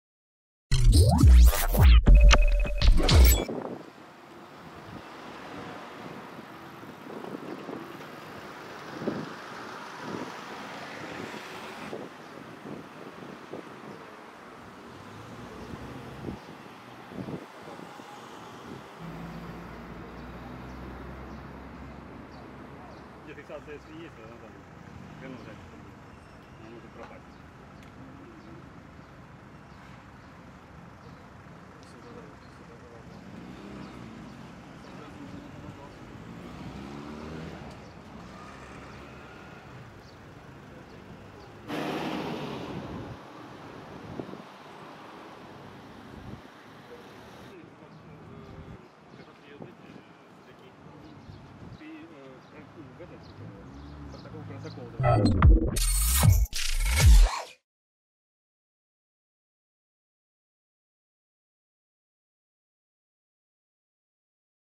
В Днепре на Титова столкнулись ВАЗ и Mitsubishi: двое пострадавших
В субботу, 2 июля, на улице Титова столкнулись автомобили ВАЗ и Mitsubishi. В ДТП пострадали водитель и пассажир ВАЗ, с места аварии их забрала скорая.